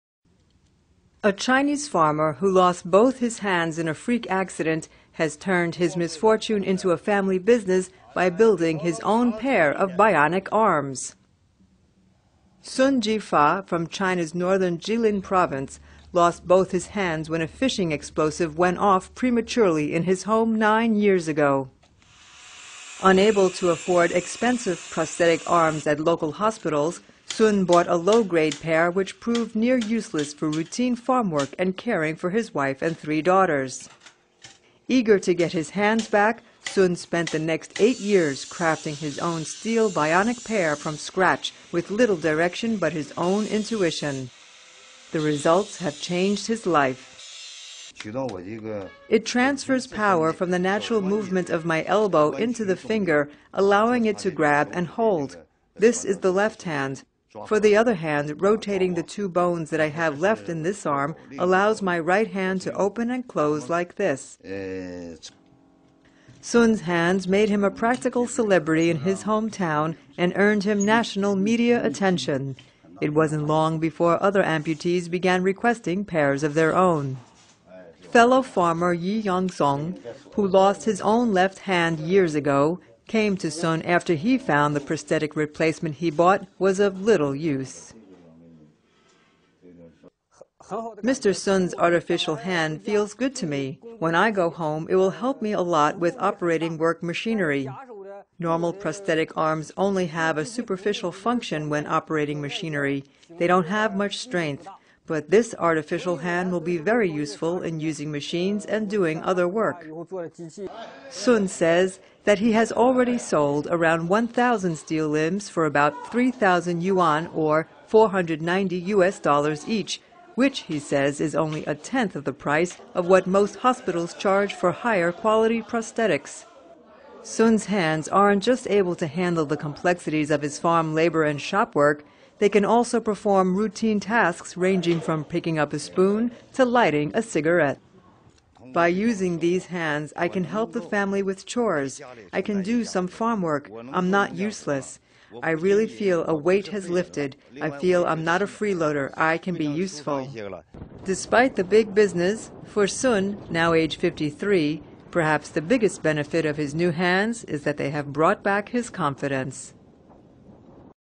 مزارع صيني يصنع لنفسه أيادي صناعية
المزارع الصيني فقد يديه الثنتين في حادث ولكن لعدم مقدرته على تكاليف العلاج والايدي الاصطناعية الطبيةـ قرر ان يصنعها بنفسه!